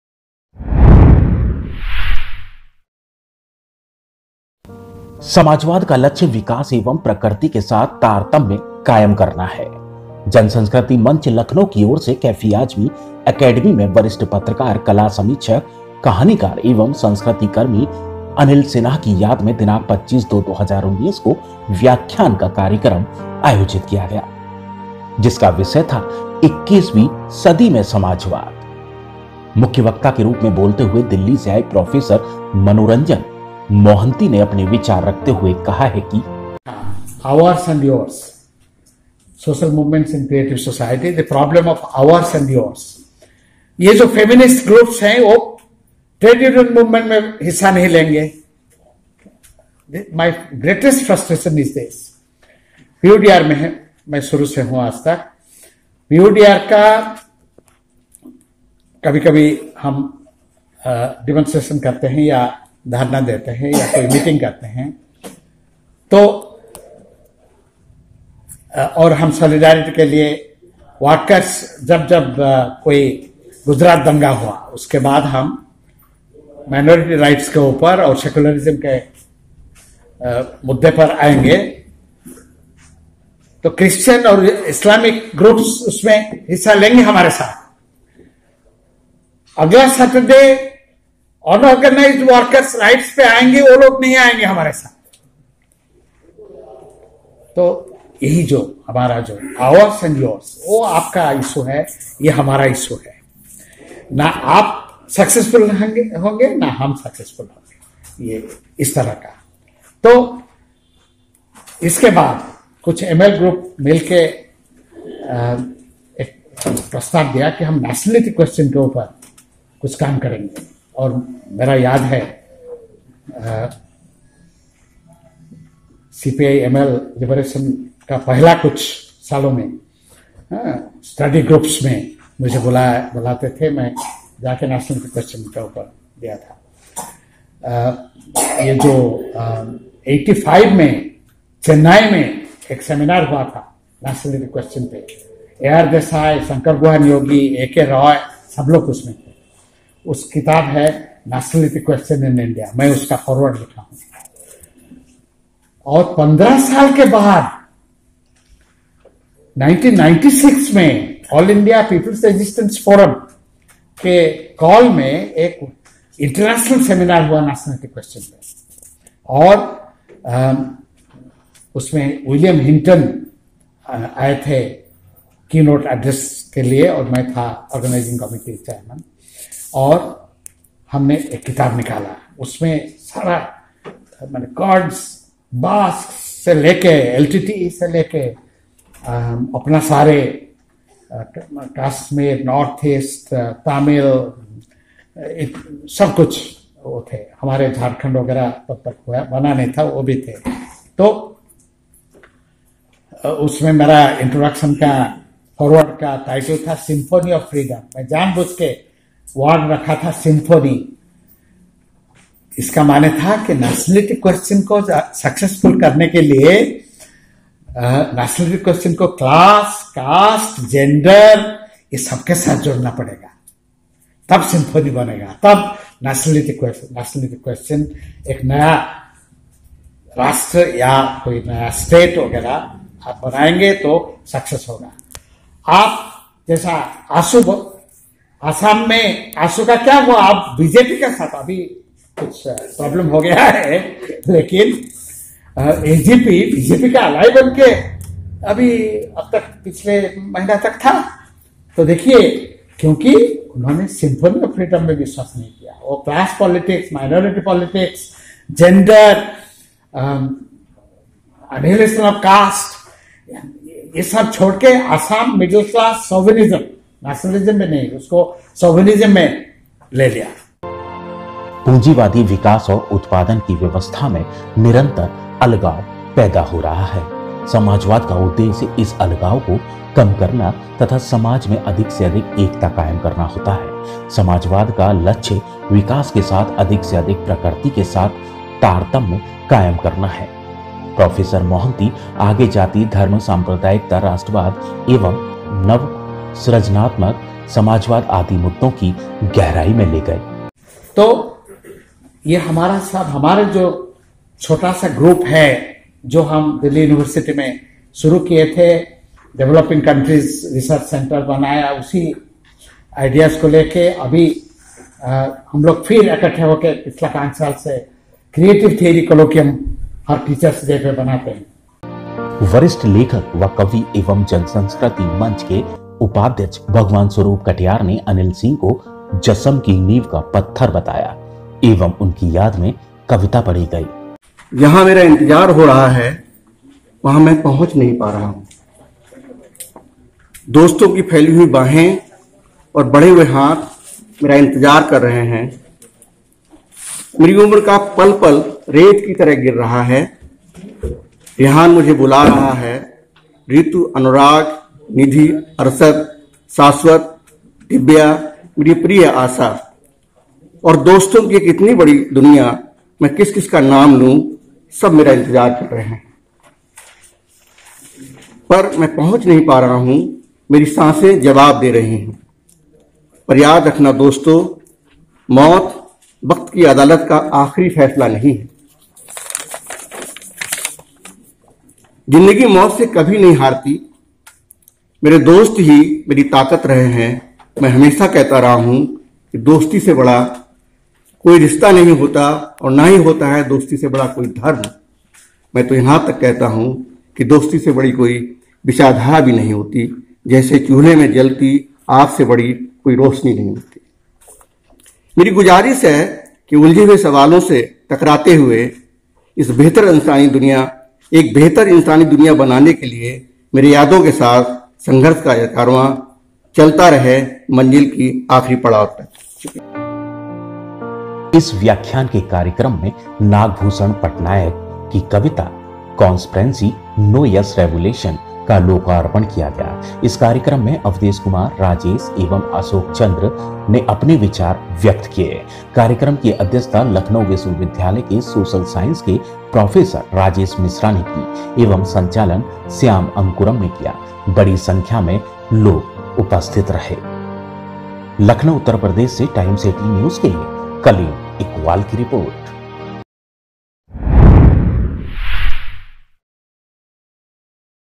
समाजवाद का लक्ष्य विकास व प्रकृति के साथ तारतम्य कायम करना है
रिपोर्ट कलीम इक़बाल